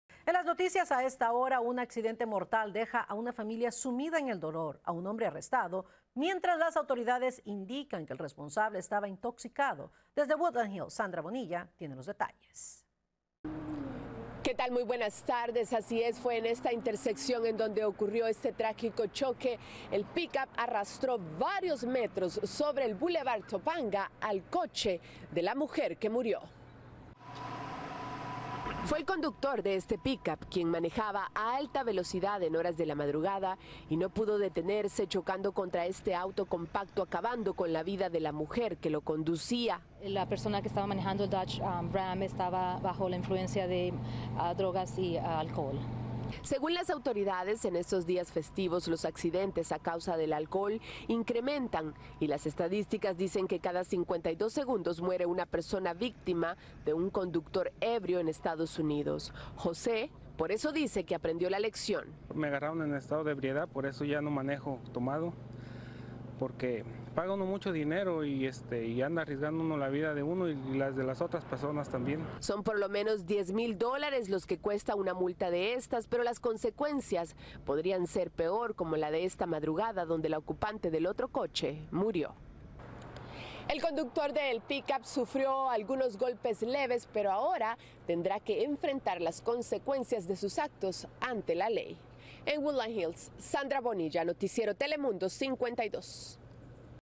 Muere una mujer en accidente en Woodland Hills
Una mujer que conducía un pequeño sedán murió esta madrugada en un choque con una camioneta en Woodland Hills, dijeron las autoridades.

El accidente ocurrió como a la 1:47 a.m., en la intersección del Bulevar Topanga Canyon con la Calle De La Osa, informó el Departamento de Policía de Los Ángeles.

La mujer de unos 50 años de edad fue declarada muerta en el lugar, dijo la policía.

El conductor de la camioneta resultó herido, según informes de la escena.

La Policía de Los Ángeles investiga el accidente.